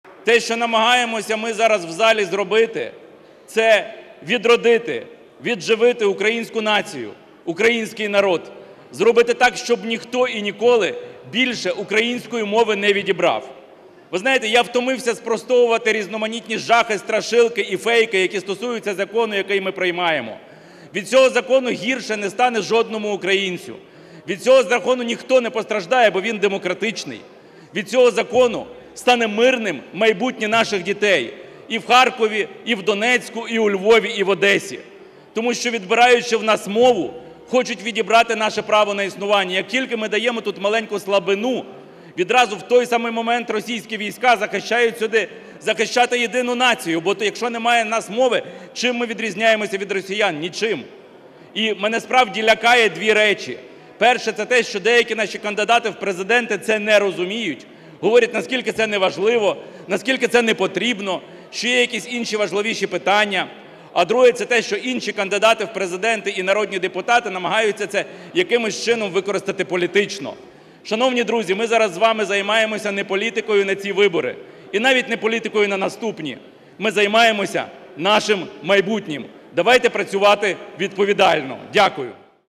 Ми повинні зробити так, щоб ніхто й ніколи не відібрав в українців їх мову, – Микола Княжицький
Необхідно зробити все так, щоб більше ніхто й ніколи не відібрав в українського народу його мову і не заважав українцям відроджувати свою націю.

Про це заявив народний депутат фракції "Народний фронт", голова Комітету з питань культури і духовності Микола Княжицький під час виступу з парламентської трибуни, продовжуючи розгляд поправок до мовного законопроекту.

"Те, що ми зараз намагаємось зробити в залі – це відродити, відживити українську націю, український народ. Зробити так, щоб ніхто й ніколи більше української мови не відібрав", – заявив він.

"Я вже стомився спростовувати різноманітні жахи, страшилки і фейки щодо закону, який ми розглядаємо. Від цього закону не стане гірше жодному українцю, від нього ніхто не постраждає, бо він – демократичний. Від цього закону стане мирним майбутнє наших дітей і в Харкові, і в Донецьку, і у Львові, і в Одесі", – зазначив народний депутат.

"Відбираючи в нас мову, у нас хочуть відібрати право на існування. Щойно ми даємо маленьку слабину, в той самий момент російські війська йдуть сюди "захищати єдину націю". Бо якщо в нас немає мови, то чим ми відрізняємось від росіян? Нічим", – пояснив представник "Народного фронту".

"Мене справді лякають дві речі. Перша – це те, що деякі наші кандидати в президенти цього не розуміють: говорять, наскільки це неважливо, непотрібно, що є якісь важливіші питання. А друге – це те, що інші кандидати в президенти та народні депутати намагаються це якимось чином використати політично. Шановні друзі, ми зараз займаємося не політикою на ці вибори, і навіть не політикою на наступні – ми займаємося нашим майбутнім. Давайте працювати відповідально", – підсумував Княжицький.

Приєднуйся!